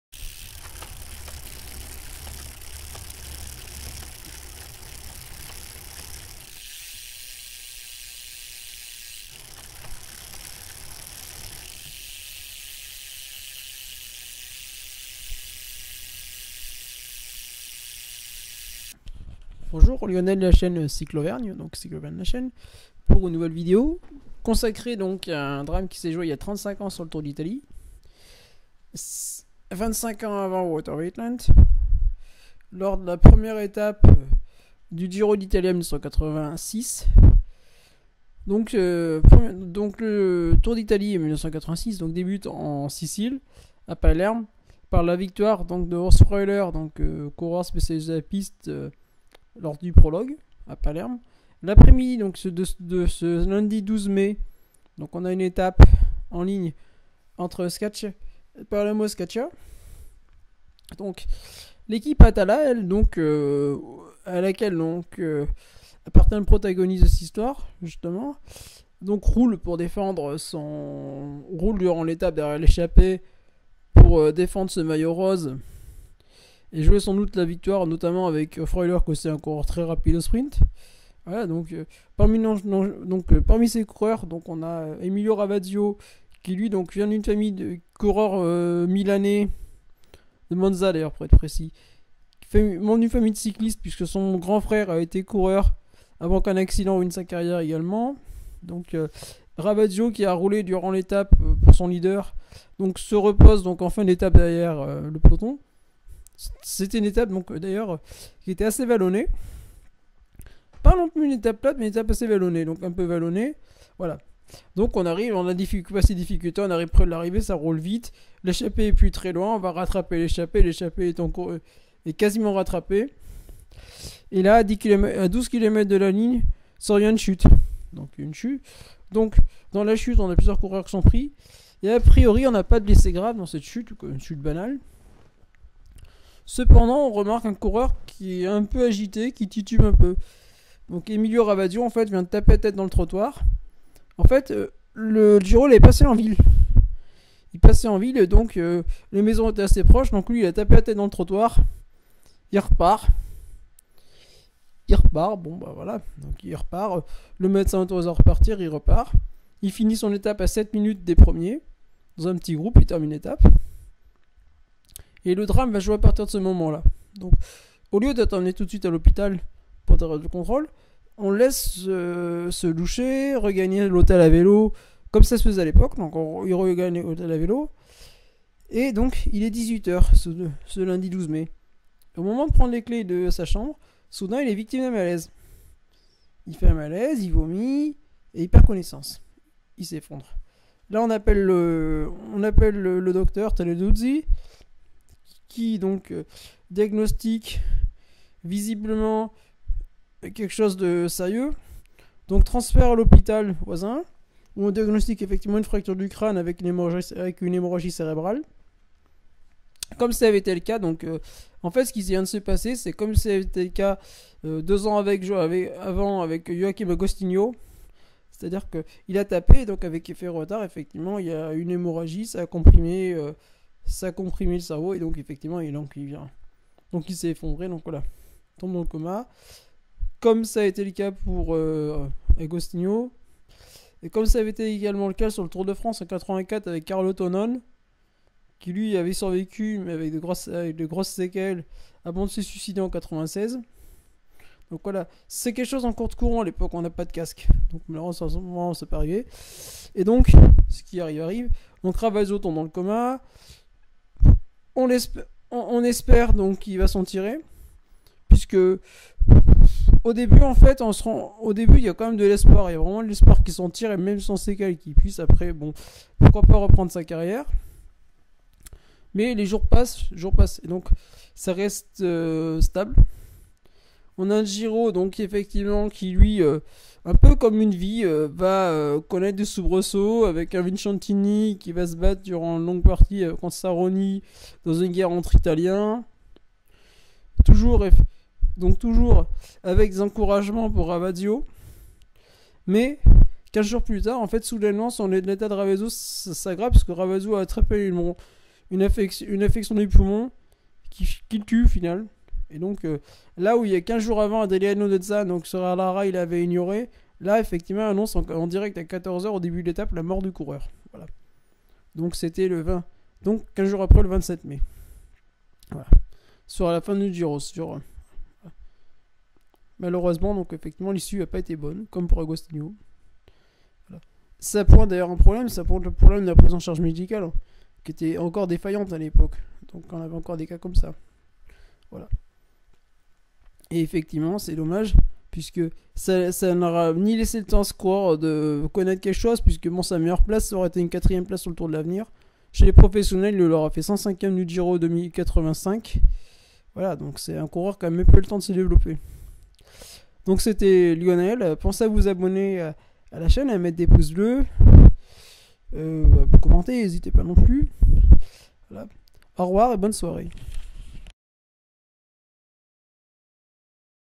Emilio Ravasio
Emilio Ravasio était un jeune coureur italien, né en 1962, il entamé sa seconde saison et son second Giro lors de cette première étape du Giro, le 12 mai 1986 en Sicile, quand il fut victime d'un chute, percutant un muret tête première, il repart et rallie la ligne avec 7 minutes sur le peloton principal. Mais une heure plus tard, il s'effondre victime d'un traumatisme crânien et d'une hémorragie cérébrale qui lui seront fatals 15 jours plus tard.

Source : Gazzeta dello Sport : 27/05/1986.